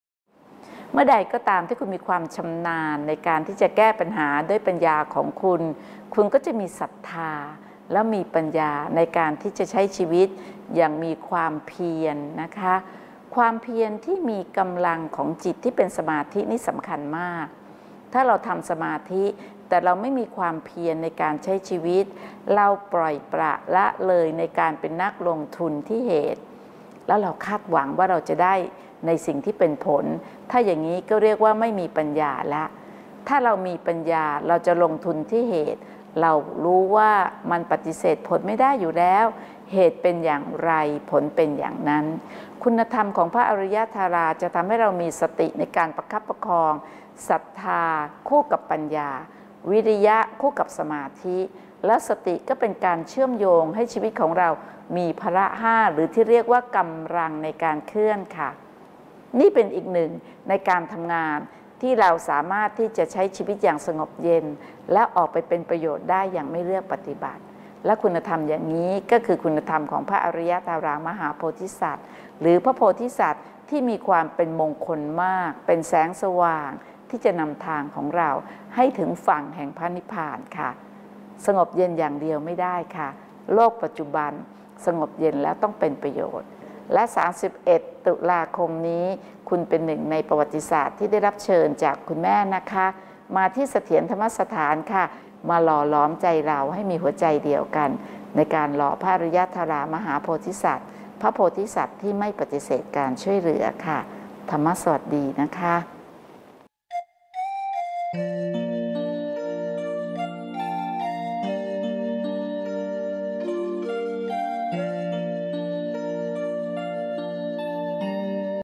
มีศรัทธาและปัญญา
ความเพียรที่มีกำลังของจิตที่เป็นสมาธิเป็นสิ่งสำคัญ

หากเราทำสมาธิแต่เราไม่มีความเพียร

นั่นคือเราขาดปัญญา

มีศรัทธาและปัญญา คุณธรรมพระอารยตารามหาโพธิสัตว์ พระโพธิสัตว์ที่ไม่เคยปฏิเสธการช่วยเหลือ

พฤหัสที่ 31 ตุลาคมนี้ ขอเชิญลูกศิษย์กลับบ้านร่วมงานกตัญญูท่านแม่ชีศันสนีย์ เสถียรสุต พร้อมร่วมพิธีเททองหล่อองค์พระอารยตารามหาโพธิสัตว์

รายละเอียดสามารถติดตามได้ทางหน้าเพจเสถียรธรรมสถาน